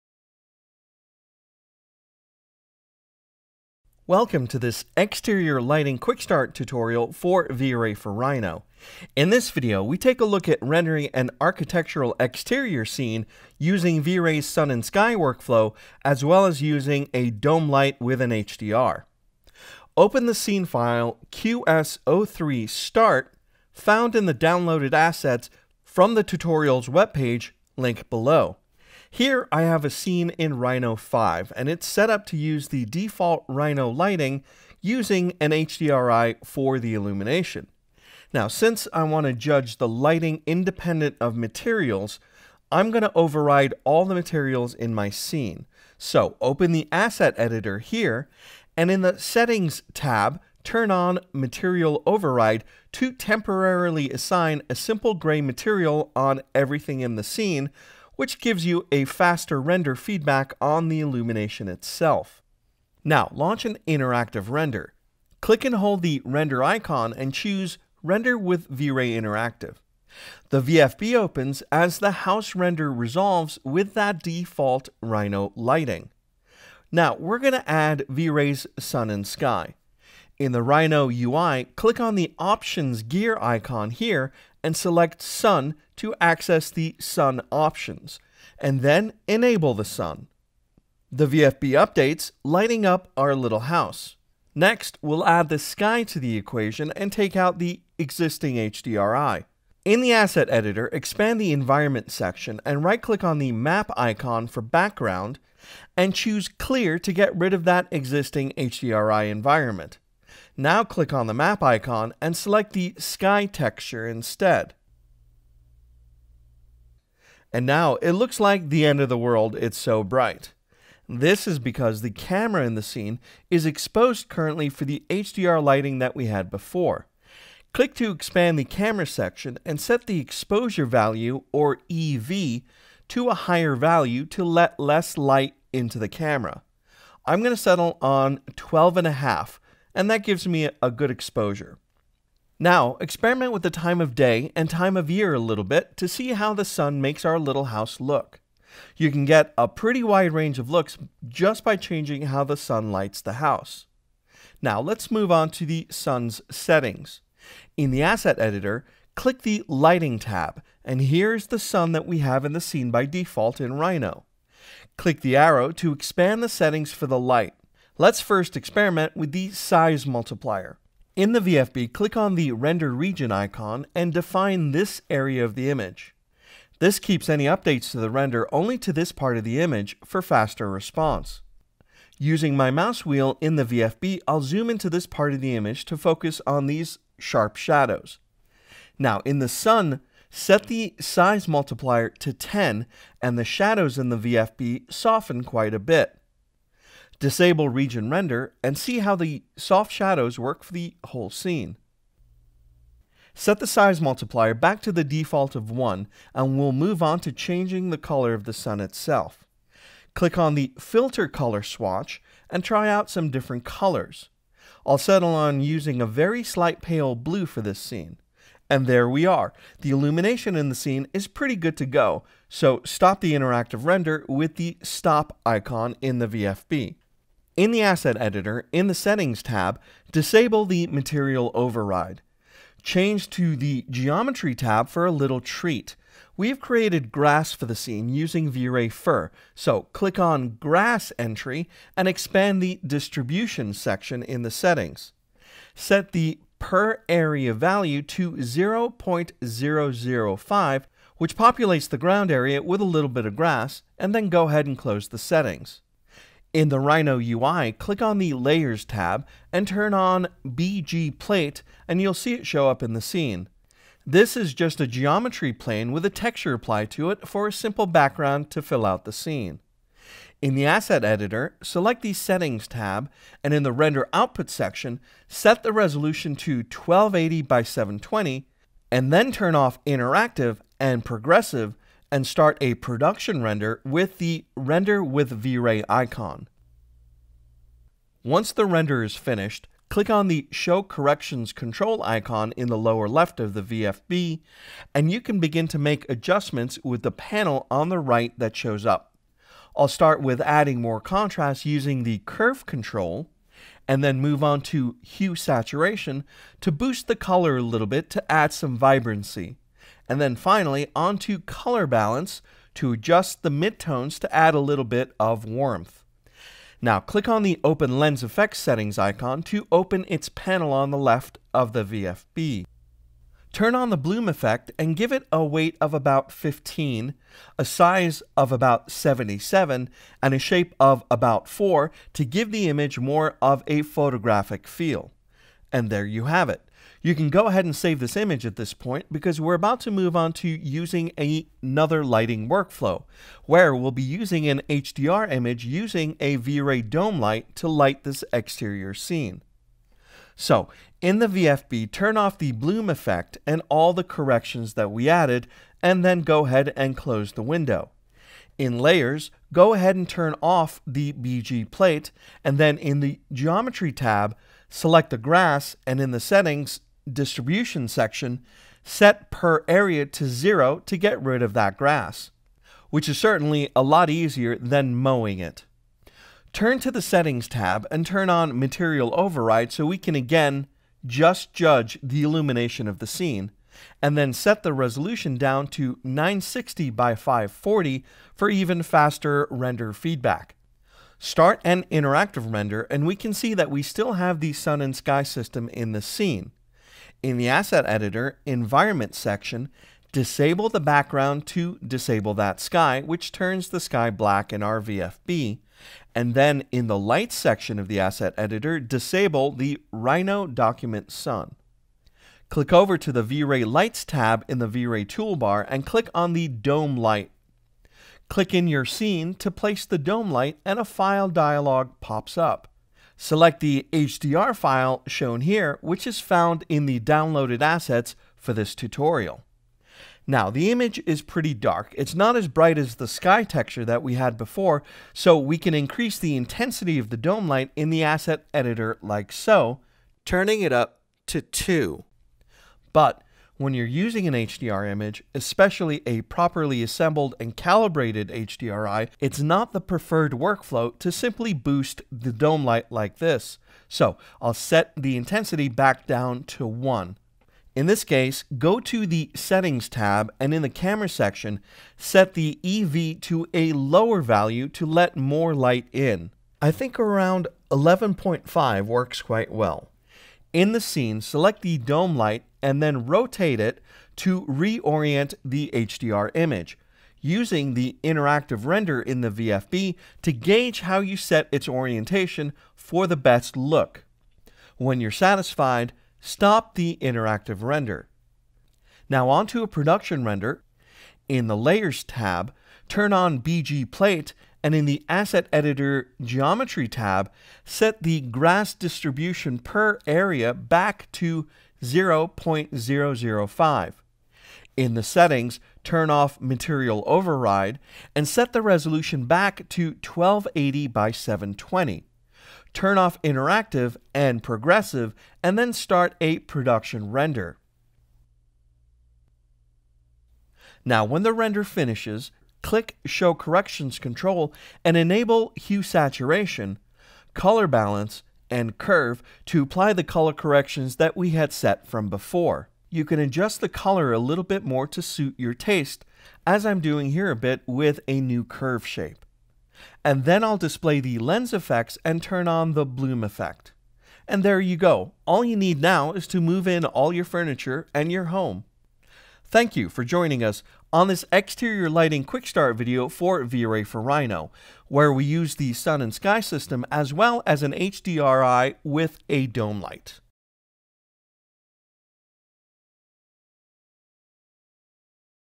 V-Ray for Rhino – Quick Start: Exterior Lighting
This video covers lighting techniques for exteriors.  We take a look at rendering an architectural exterior using both V-Ray’s Sun and Sky System and Image Based Lighting with a High Dynamic Range Image on a Dome light.